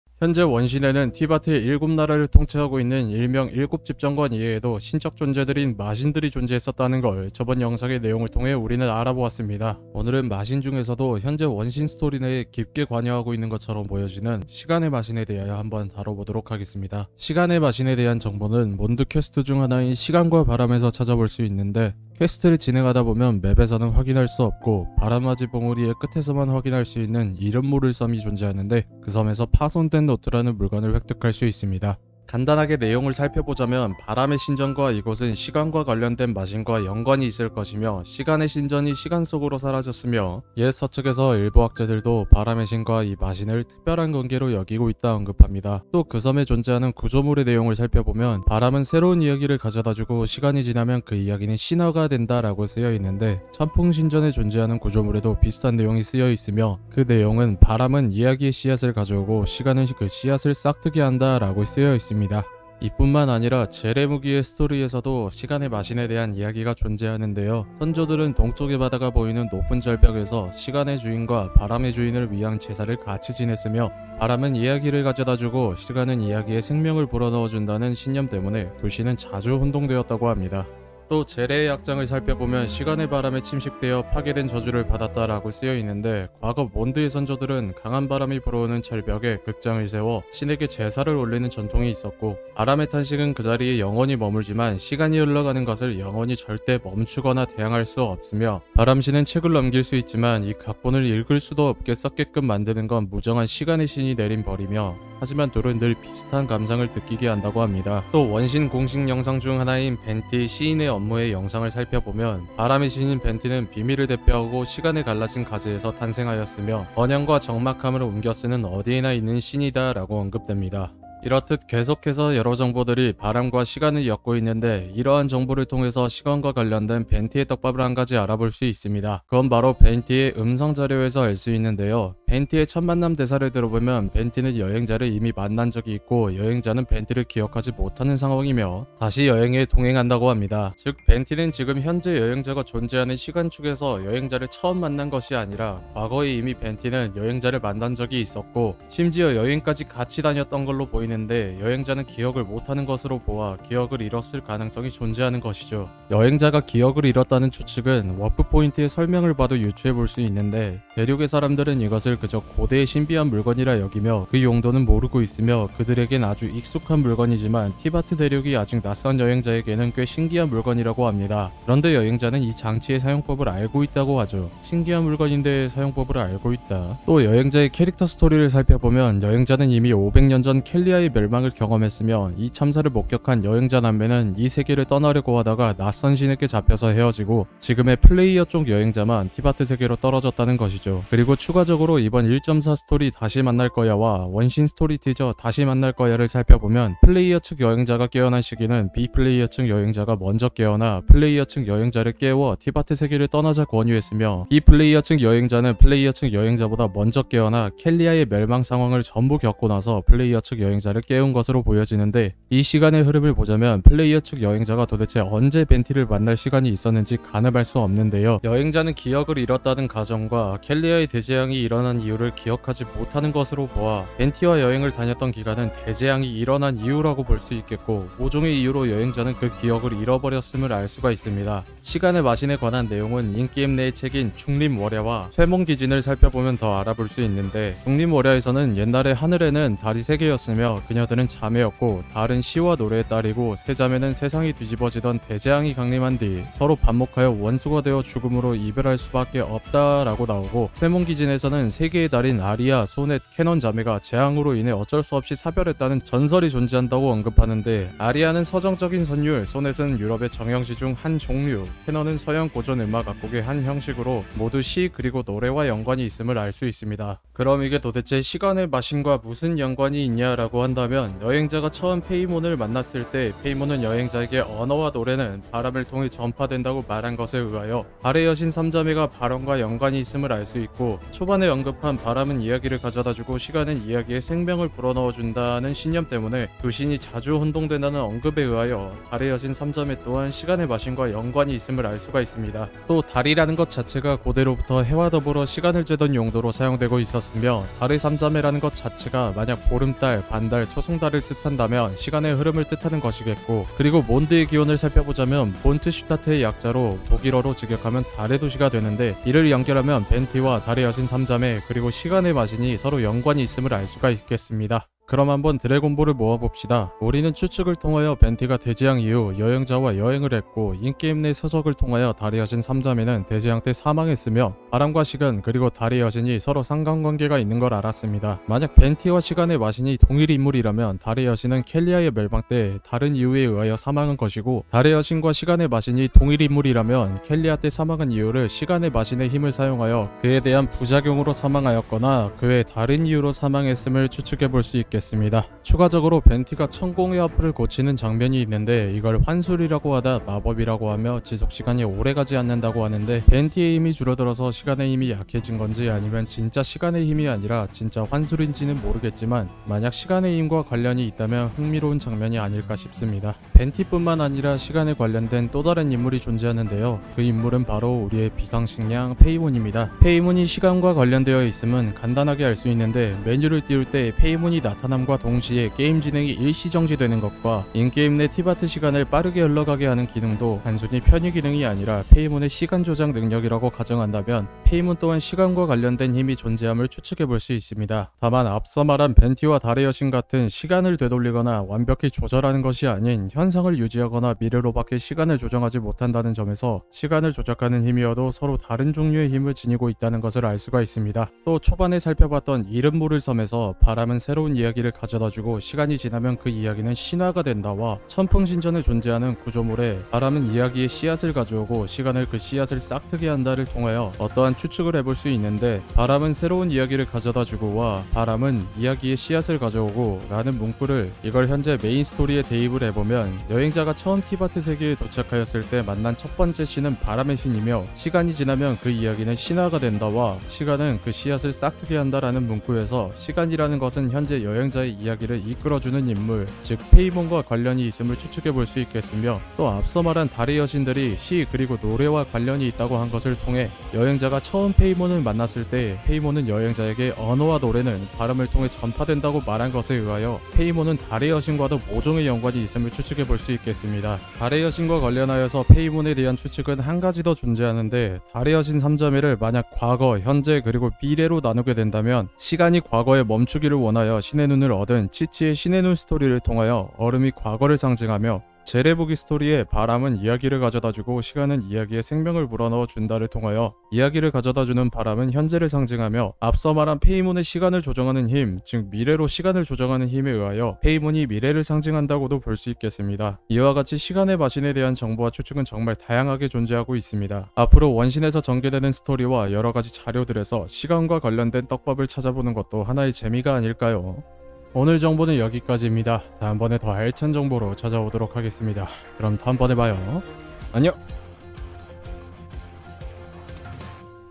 원신 떡밥덩어리 시간의 마신에 대하여
00:17 시작
02:01 벤티와의 연관성
03:51 서적 그리고 달의 여신
05:15 드래곤볼
06:03 제 2의 인물
08:03 마무리

영상 업로드가 늦어져서 다시 한번 죄송합니다

본 내용은 전부 뇌피셜입니다

영상 자료 출저